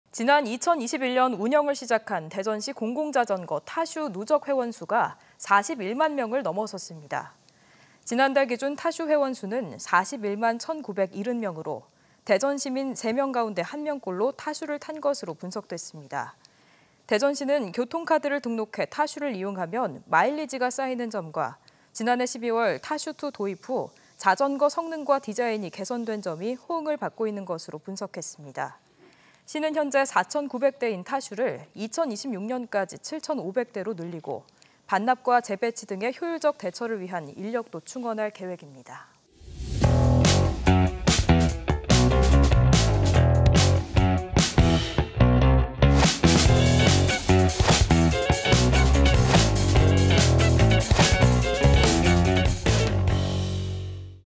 '타슈' 회원 41만명 돌파..시민 3명 가운데 1명 이용 | TJB 자막뉴스
지난 2021년 운영을 시작한
대전시 공공자전거 '타슈' 누적 회원 수가
41만명을 넘어섰습니다.